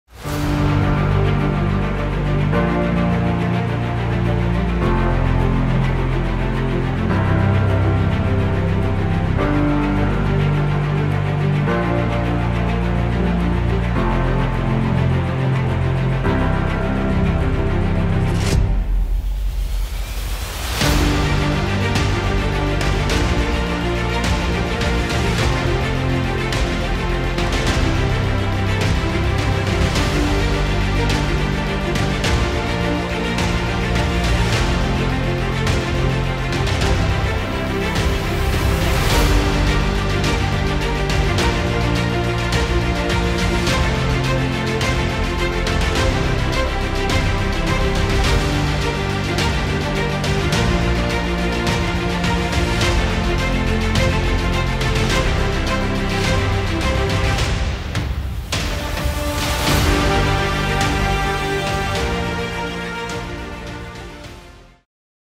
Berkshire Hathaway HomeServices Timeline Video
This video highlights the number of network agents, offices, transactions, and volume since the inception of Berkshire Hathaway HomeServices in September 2013.